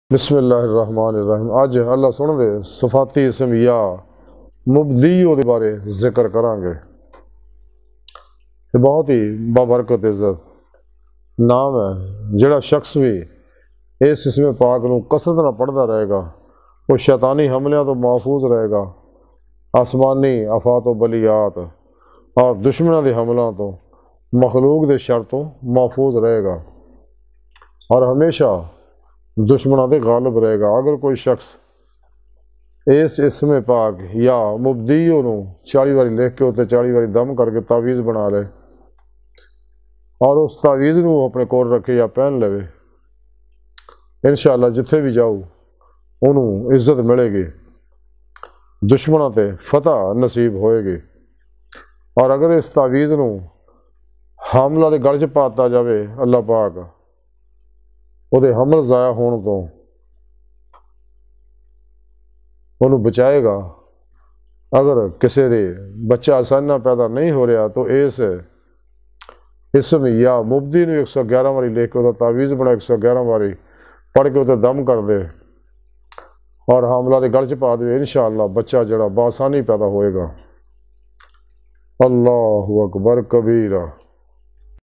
اسم یا مُبدیُّ کے عمل سے آفات و بلیّات شیطانی حملوں دشمن کے شر سے حفاظت
روحانی علاج